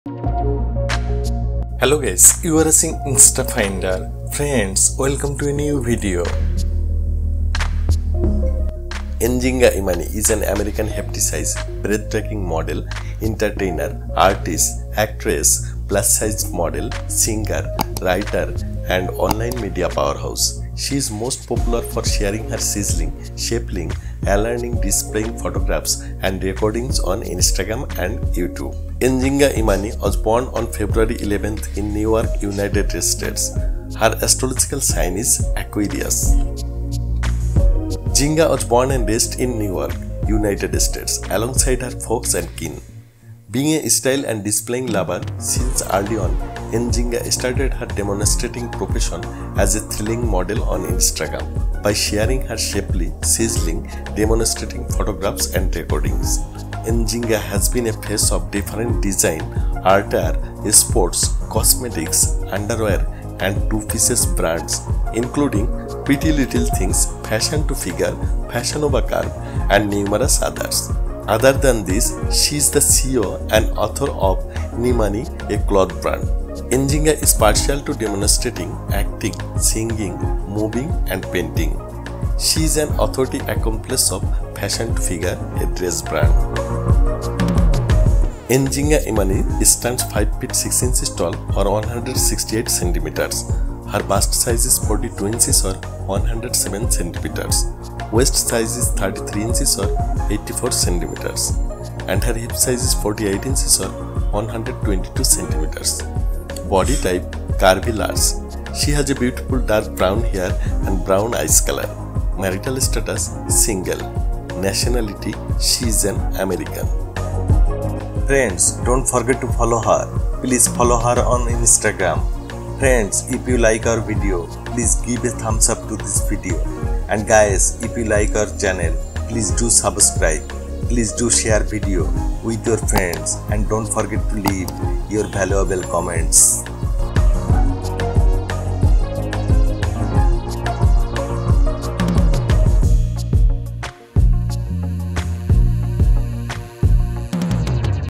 Nzinga Imani 🇺🇸...| Plus Size Model | Curvy Fashion Model | Brand Ambassador | Lifestyle,Biography2
Original content owner: @Nzinga Imani
Follow on Instagram - @nzingaimani

Nzinga Imani is an American hefty size, breathtaking model, entertainer, artist, Actress, Plus Model, Singer, Writer and online media powerhouse. She is most popular for sharing her sizzling, shapely, alluring, displaying photographs, and recordings on Instagram and YouTube. She is additionally renowned as the CEO of Nimani, an attire brand and an authority accomplice of Fashion to Figure, a dress line. Nzinga Imani was born in New York, United States. Her astrological sign is Aquarius. As a surprising style model, Nzinga has been a face of different design, attire, sports, cosmetics, underwear, and two-pieces brands including Pretty Little Things, Fashion to Figure, Fashion Nova Curve, and numerous others. Other than this, she is the CEO and author of Nimani, a clothing brand. Nzinga is partial to demonstrating, acting, singing, moving, and painting. She is an authority accomplice of Fashion to Figure, a dress brand.